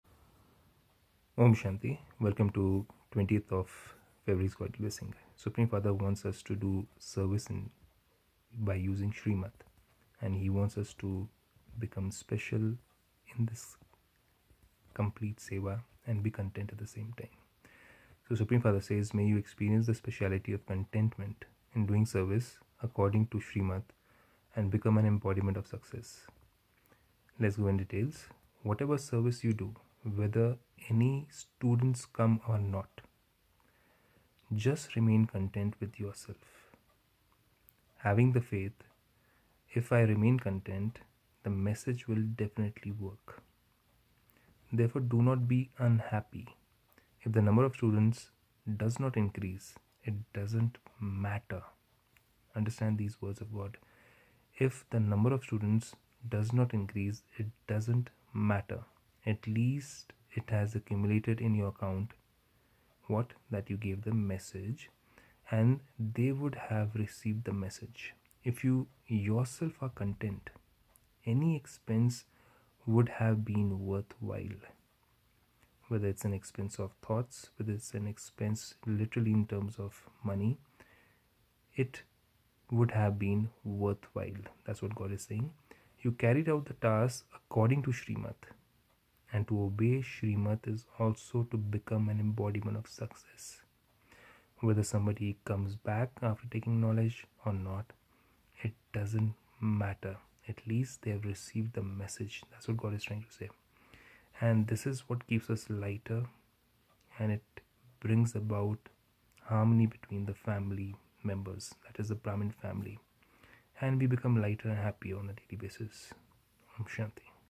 Experience the speciality of contentment in doing service according to shrimat & become an........
Day 747 May you experience the speciality of contentment in doing service according to shrimat and become an embodiment of success

Om Shanti .